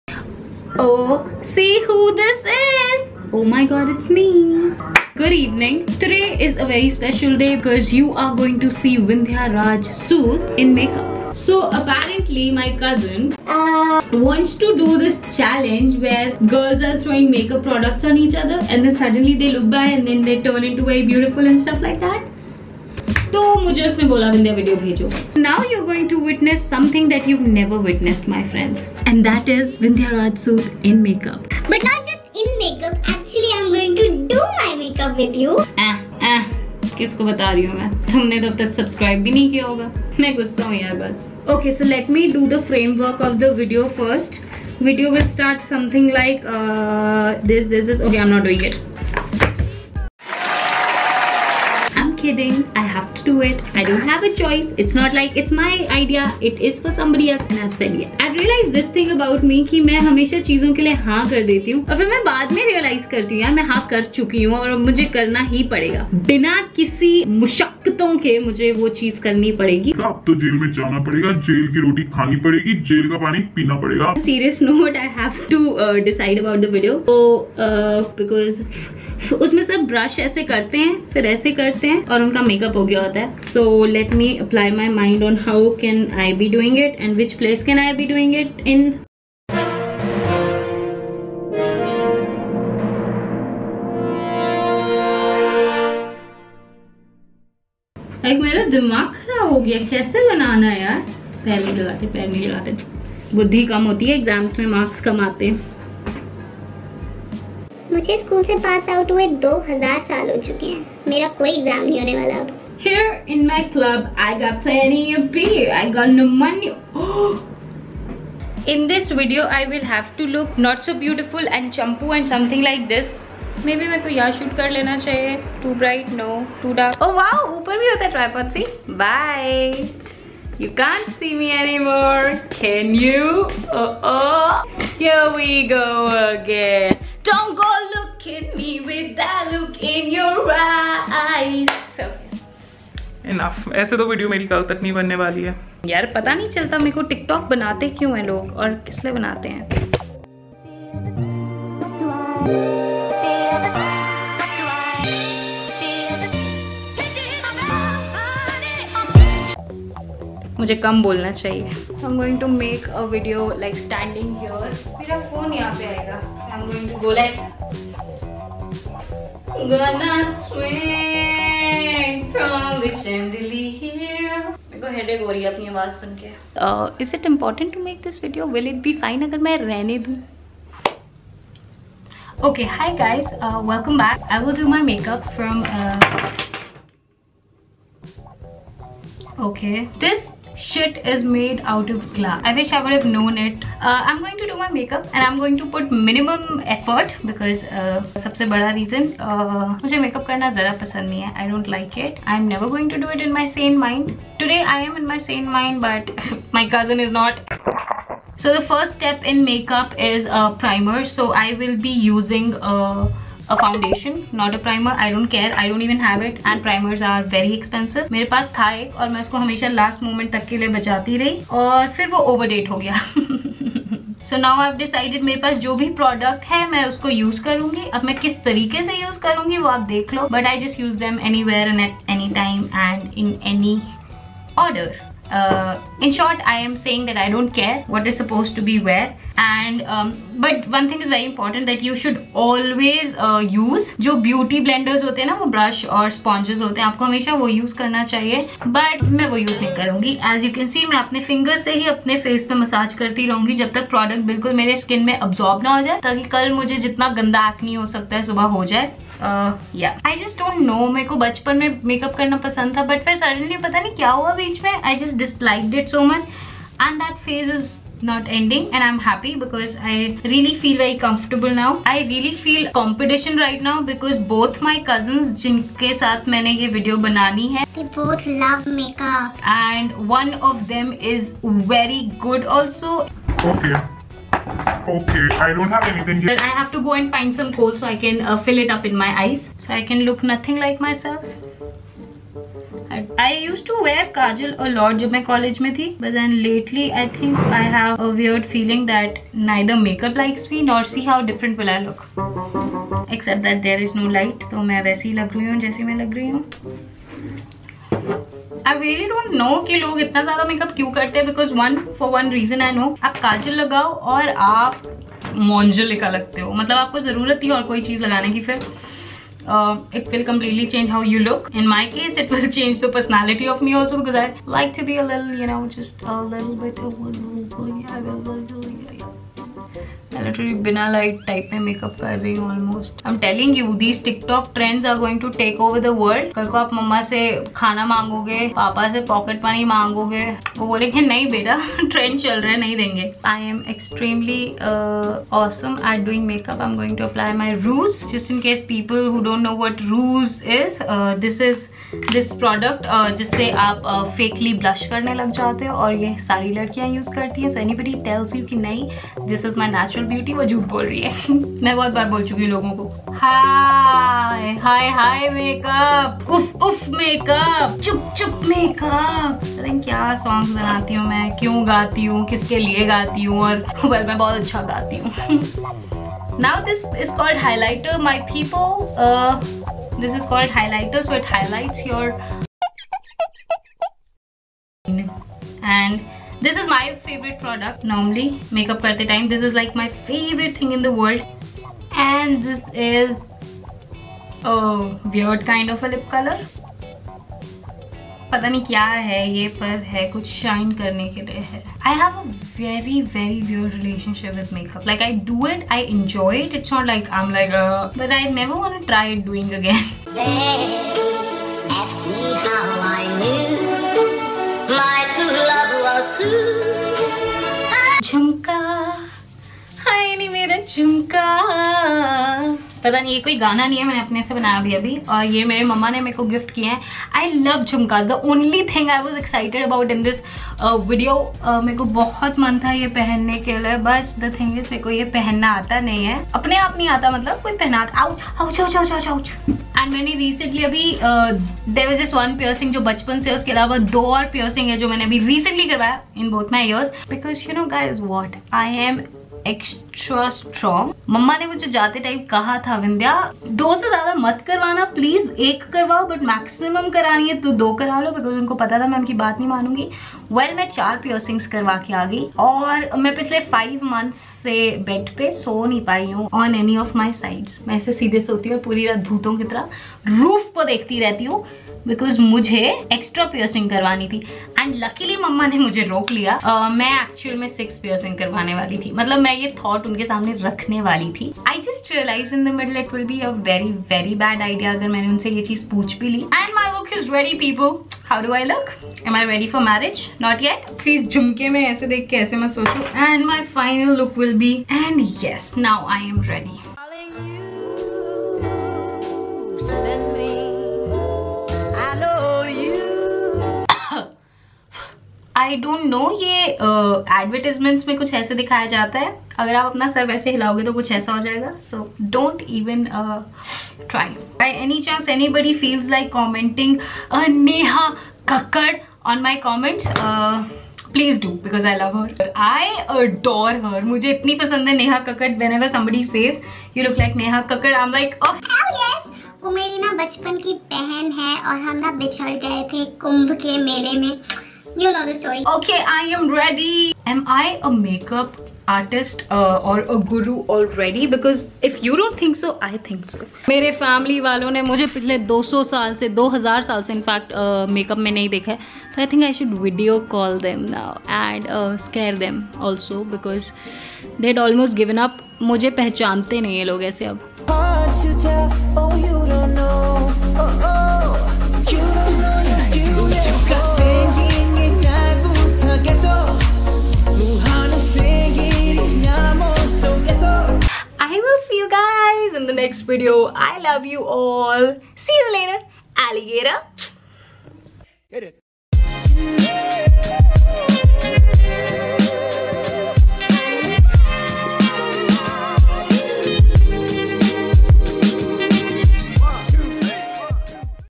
I tried a TIKTOK Challenge! BTS
I tried a Tik Tok challenge and I completely transformed myself for the video.